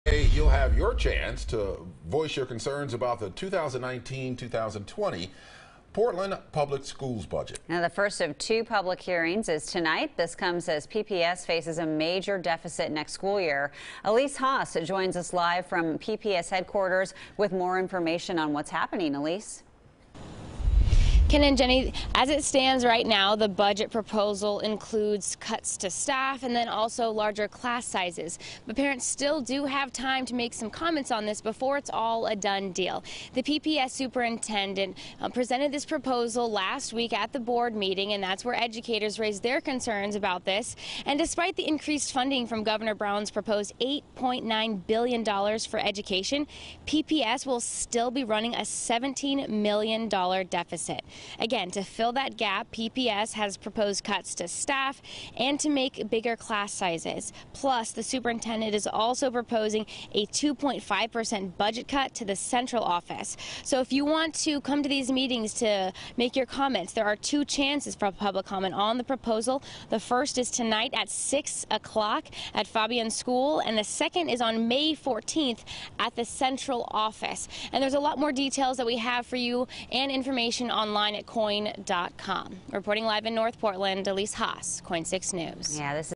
Public invited to voice concerns about PPS budget
The public will have a chance Tuesday night to voice concerns about the 2019-20 Portland Public Schools budget.